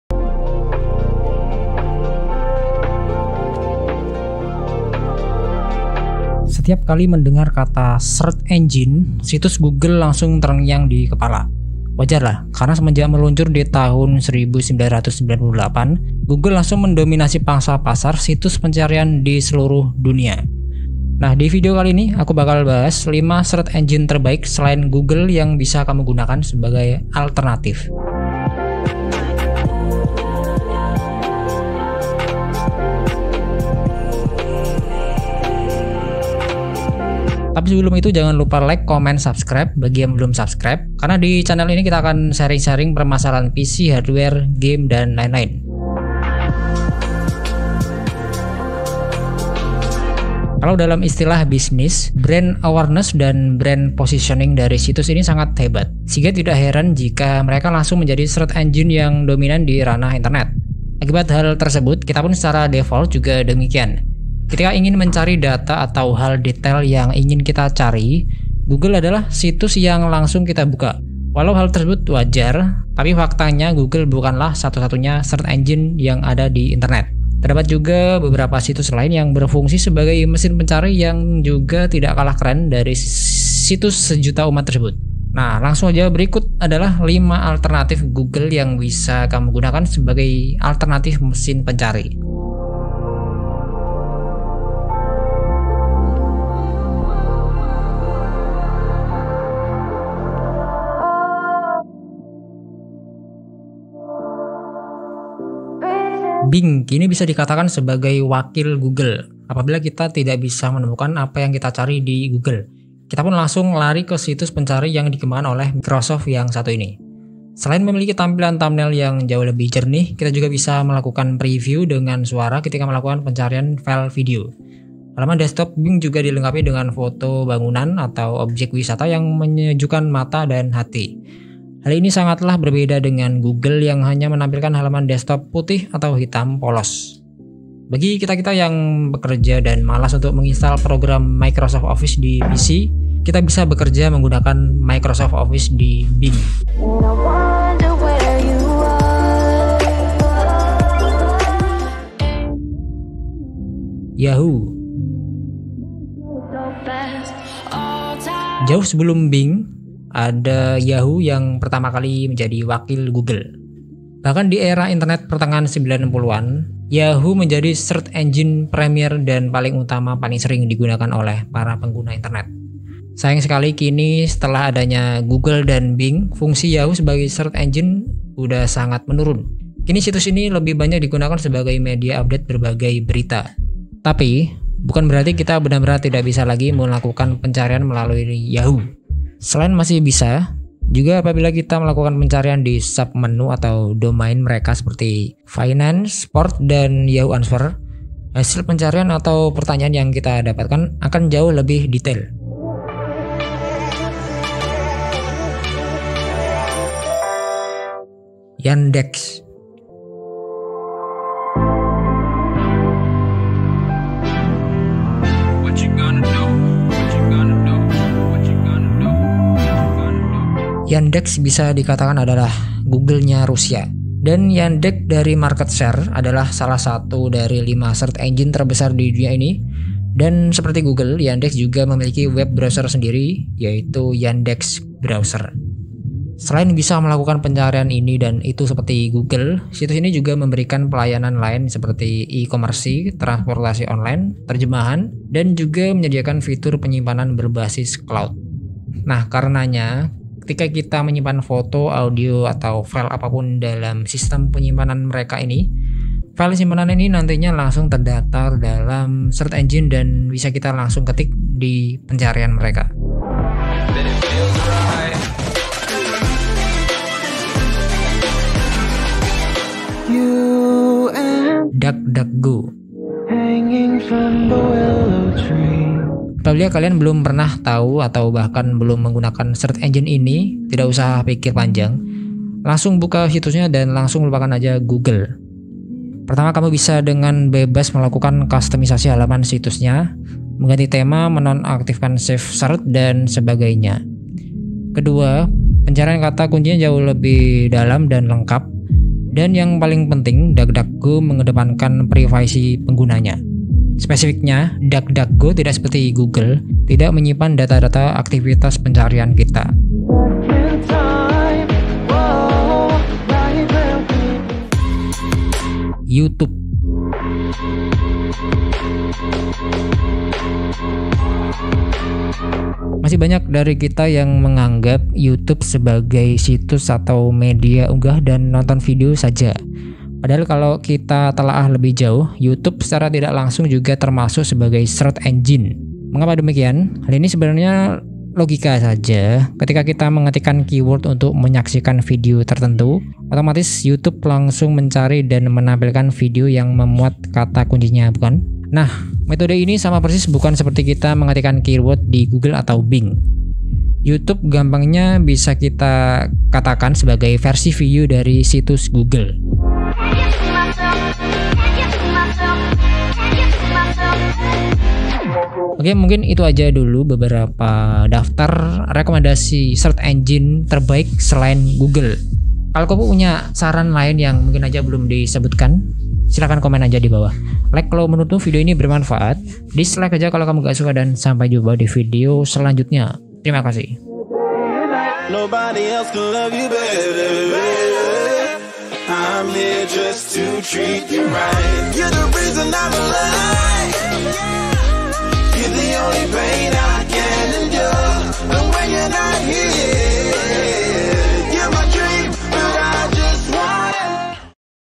Lebih Aman!! 5 Search Engine Alternatif Selain Google (mesin pencari)
Beberapa rekomendasi mesin pencari yang bisa kalian gunakan selain Google. Selengkapnya, silahkan tonton video diatas. Lebih Aman!! 5 Search Engine Alternatif Selain Google (mesin pencari)